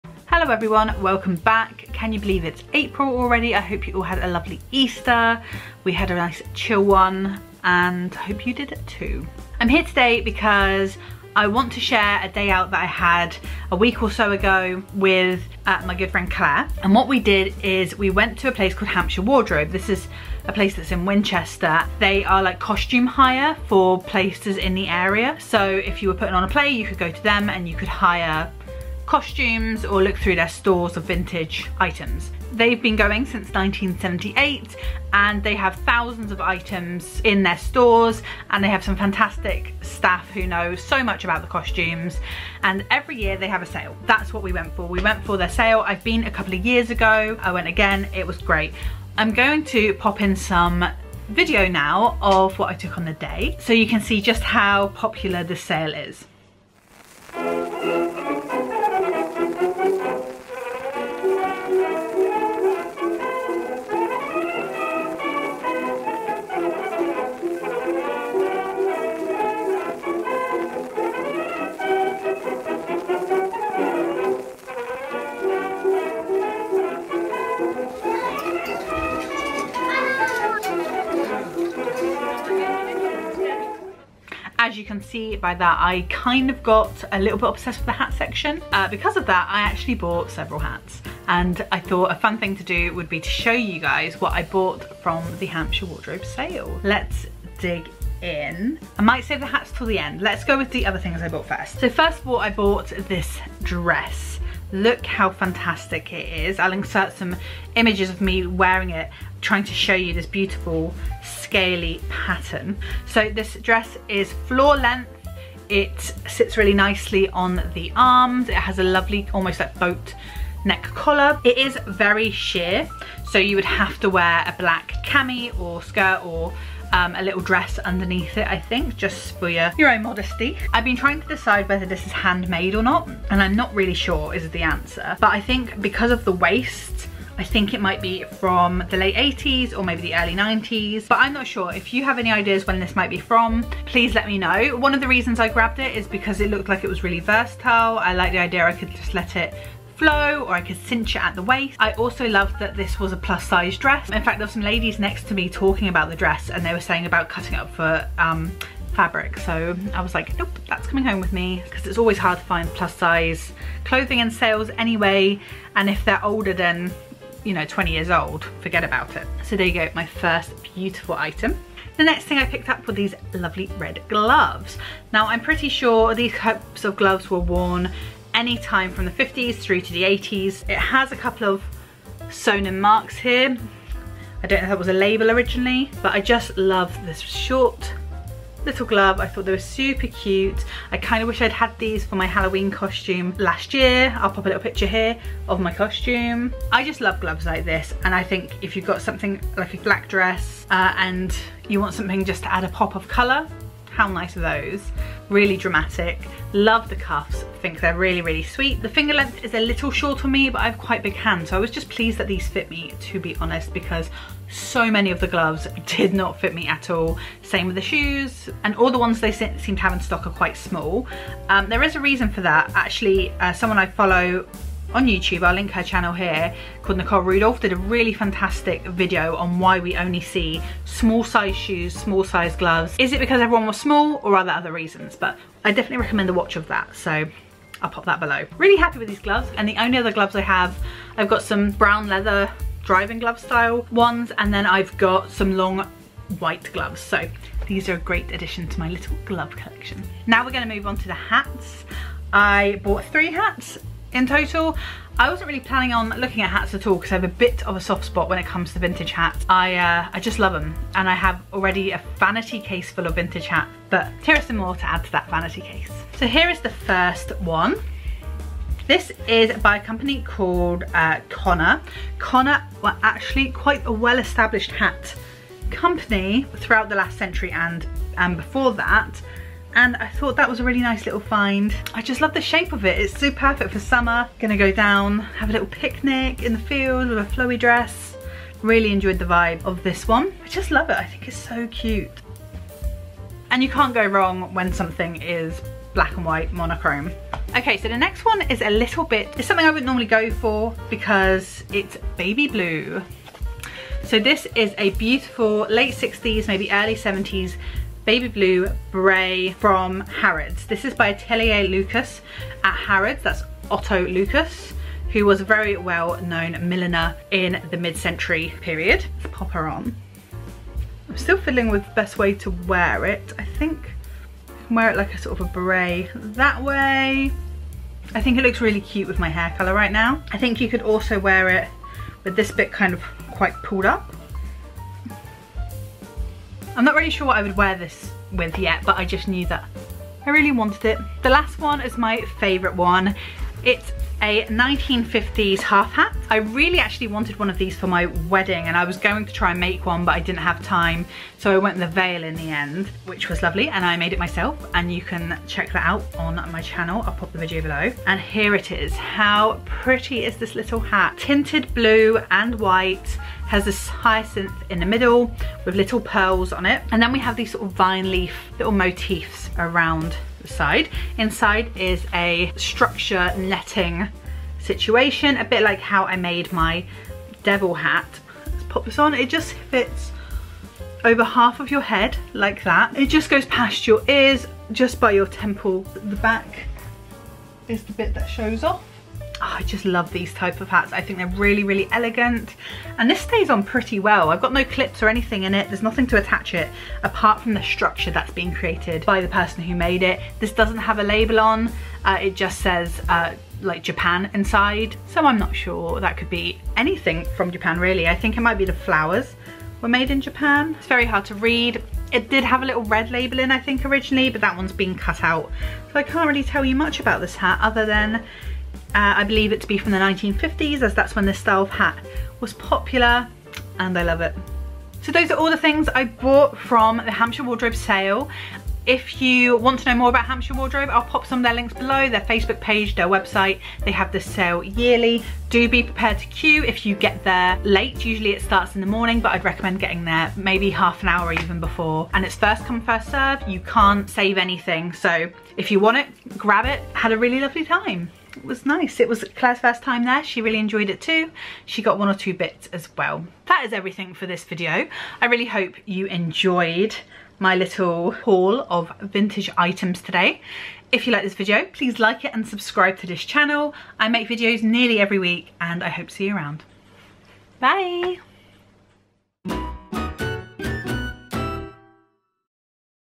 A trip to Hampshire Wardrobe | Lucy Elliott
Come with me to Hampshire Wardrobe, just on the outskirts of Winchester. In this video I go through what I bought from their glorious annual sale

Lucy x

All music is -
Purple planet
Youtube library